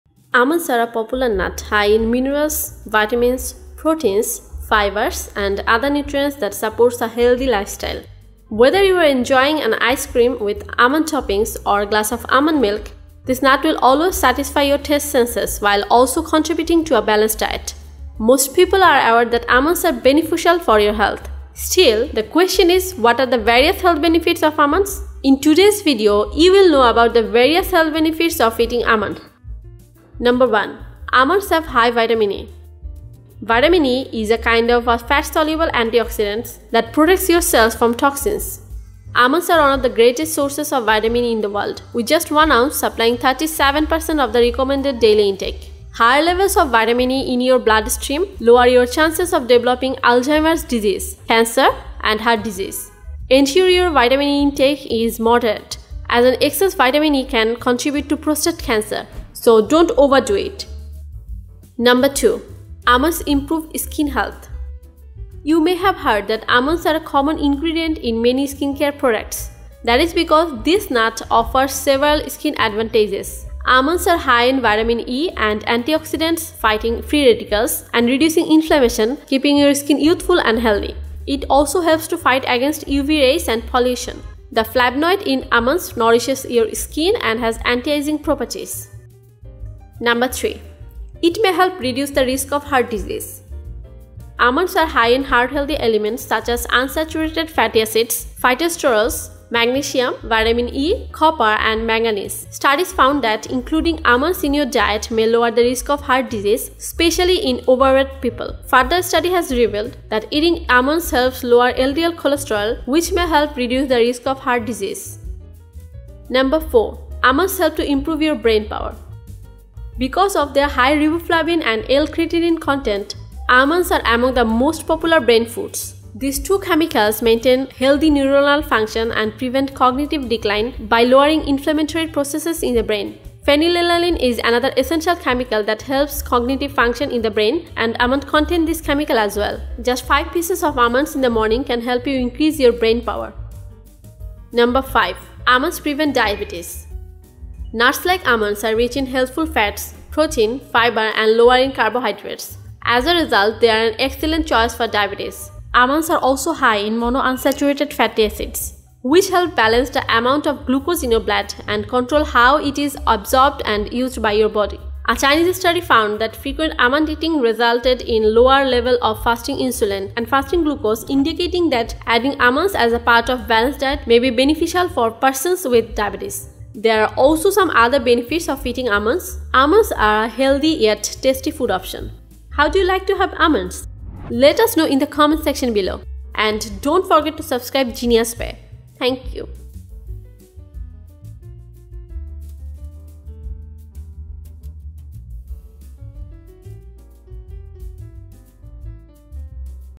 5 Amazing Health Benefits of Eating Almonds Regularly
Hey!! Welcome to GeniusBae !!

Here is the summary of the video:
Introduction to health benefits of almonds 00:00
1. Almonds have High Vitamin E 00:30
2. Almonds improve skin health. 01:01
3. It May help reduce the risk of heart disease. 01:29
4. Almonds help to Improve Your Brain Power. 01:56
5. Almonds prevent diabetes 02:24
For more information watch the video till the very end.